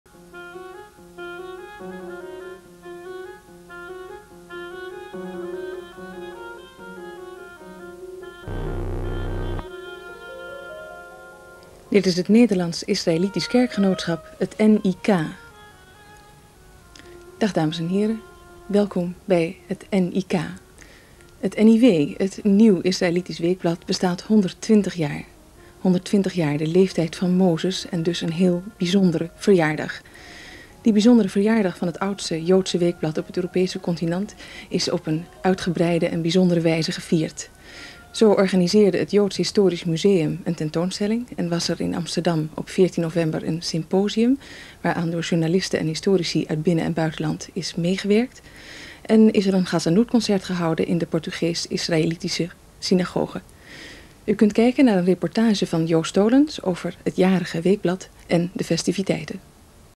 TV: NIK - Klok & Omroepster Nelleke Rademaker (19851201)
Uitgezonden op 1 december 1985.

Met dank aan Joost voor het beschikbaar stellen van deze videoband.